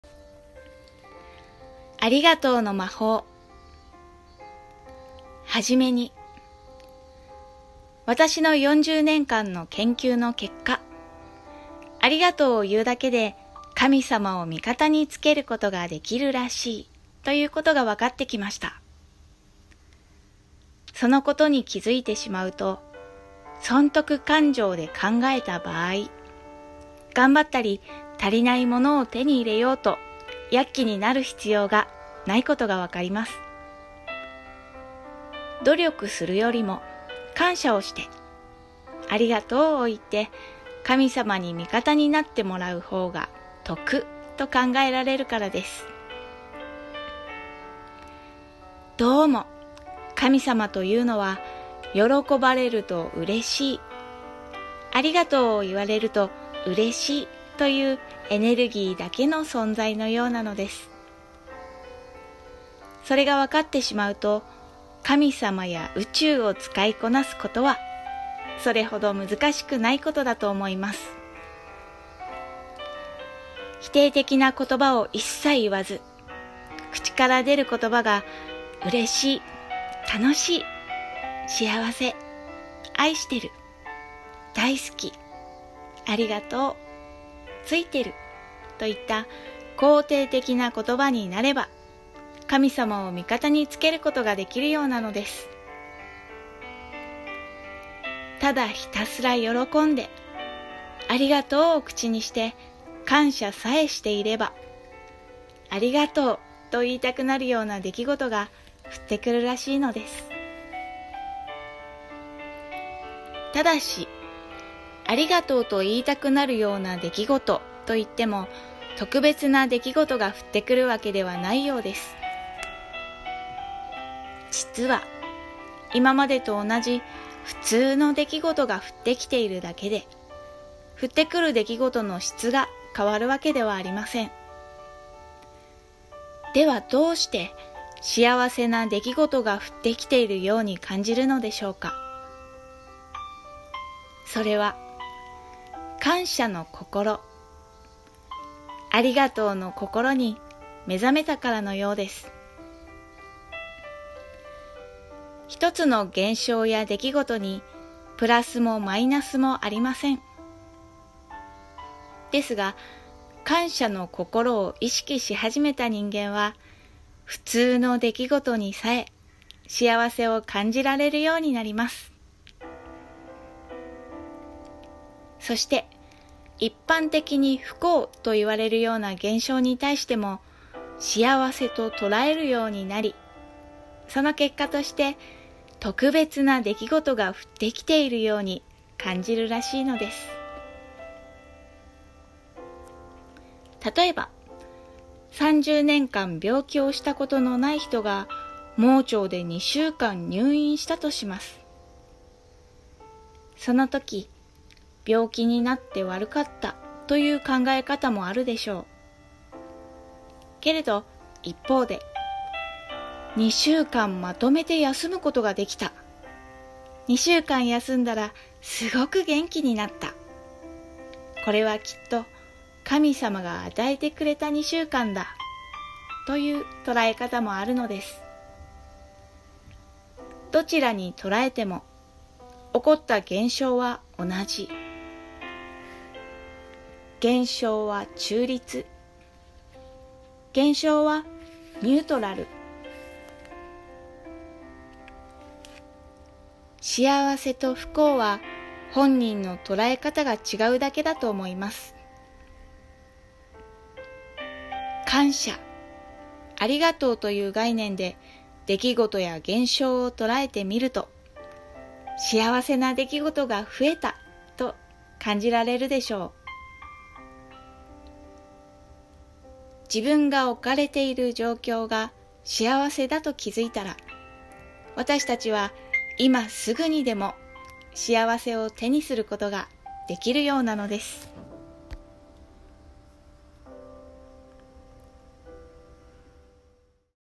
〜ありがとうの魔法〜 小林正観 《朗読》
小林正観さんの著書

『ありがとうの魔法』より

小林正観さんからのメッセージをお届けします